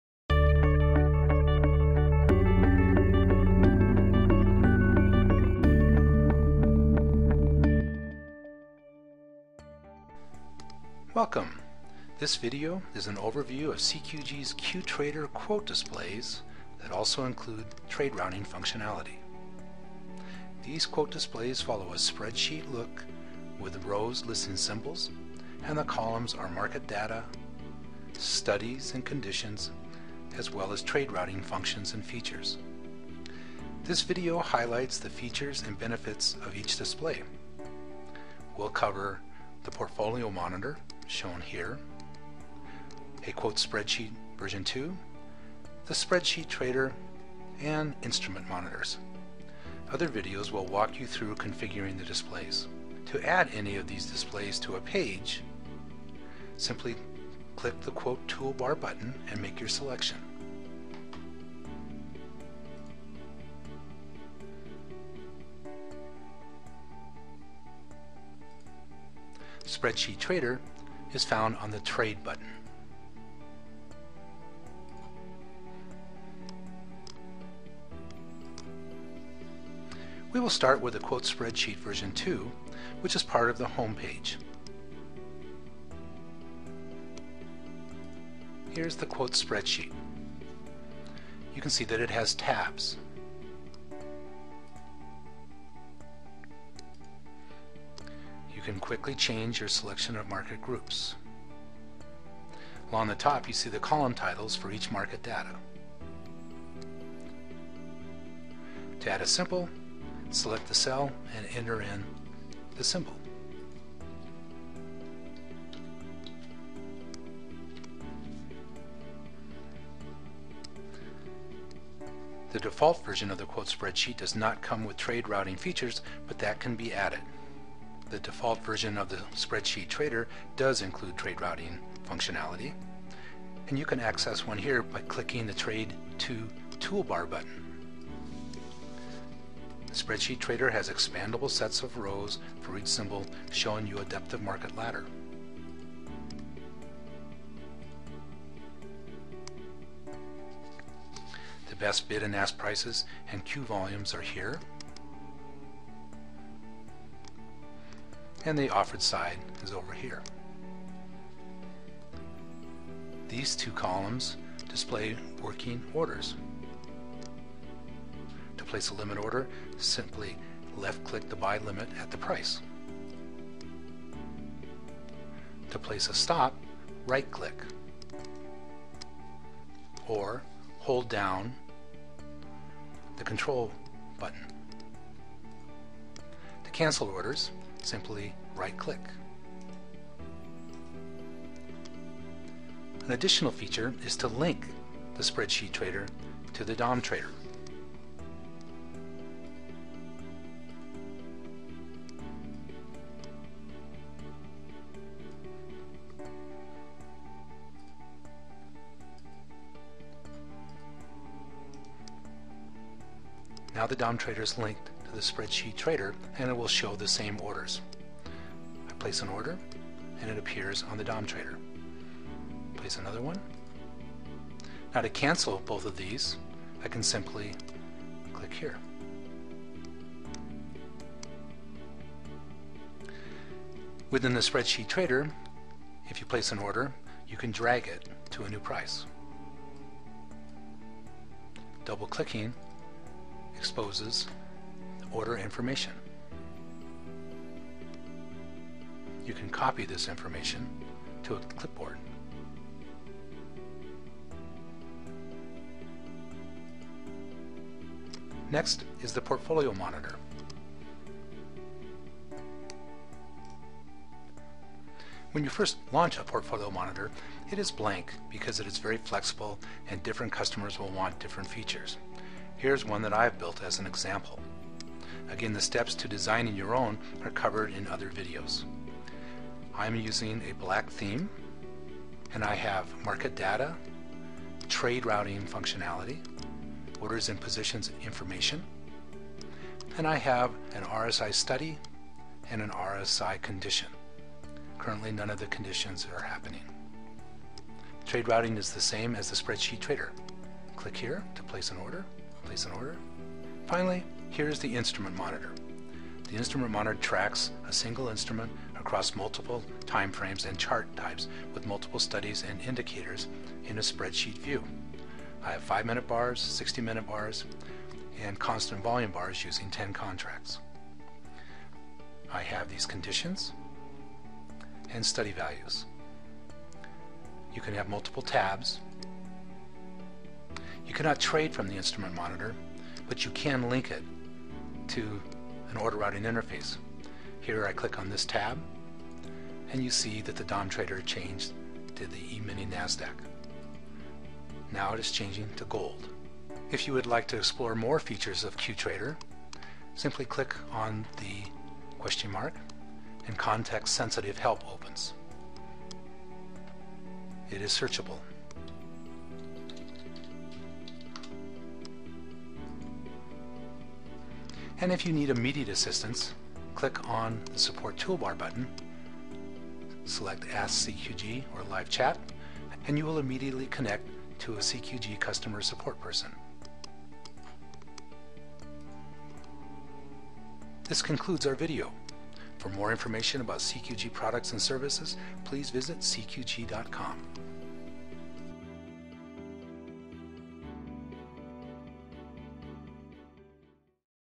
CQG QTrader Video: Quote Displays and Trade Routing
Watch this video to learn about the functionality and features of the Portfolio Monitor, the Spreadsheet Trader, and the Quote Spreadsheet Version 2.0.